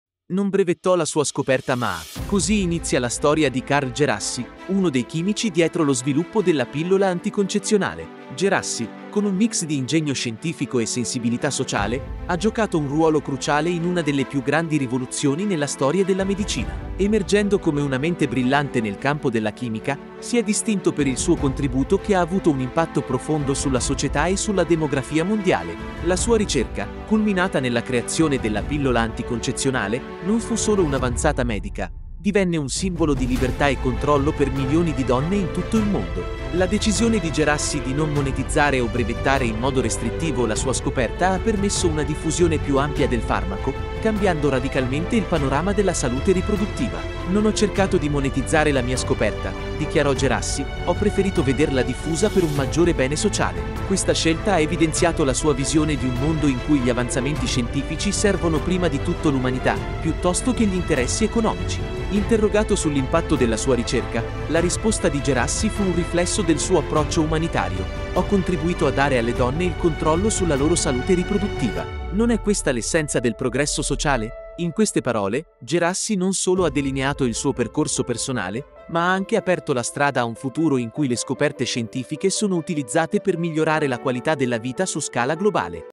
L'EROE DIMENTICATO: Carl Djerassi inventore della Pillola Anticoncezionale
Non brevettò la sua scoperta ma così inizia la storia di Carl Djerassi, uno dei chimici dietro lo sviluppo della pillola anticoncezionale. Djerassi, con un mix di ingegno scientifico e sensibilità sociale, ha giocato un ruolo cruciale in una delle più grandi rivoluzioni nella storia della medicina. Emergendo come una mente brillante nel campo della chimica, si è distinto per il suo contributo che ha avuto un impatto profondo sulla società e sulla demografia mondiale.
Leggi l'articolo completo su: ilbioss.blog

La sua ricerca, culminata nella creazione della pillola anticoncezionale, non fu solo un'avanzata medica; divenne un simbolo di libertà e controllo per milioni di donne in tutto il mondo. La decisione di Djerassi di non monetizzare o brevettare in modo restrittivo la sua scoperta ha permesso una diffusione più ampia del farmaco, cambiando radicalmente il panorama della salute riproduttiva.

"Non ho cercato di monetizzare la mia scoperta," dichiarò Djerassi, "ho preferito vederla diffusa per un maggiore bene sociale." Questa scelta ha evidenziato la sua visione di un mondo in cui gli avanzamenti scientifici servono prima di tutto l'umanità, piuttosto che gli interessi economici.

Interrogato sull'impatto della sua ricerca, la risposta di Djerassi fu un riflesso del suo approccio umanitario: "Ho contribuito a dare alle donne il controllo sulla loro salute riproduttiva. Non è questa l'essenza del progresso sociale?" In queste parole, Djerassi non solo ha delineato il suo percorso personale, ma ha anche aperto la strada a un futuro in cui le scoperte scientifiche sono utilizzate per migliorare la qualità della vita su scala globale.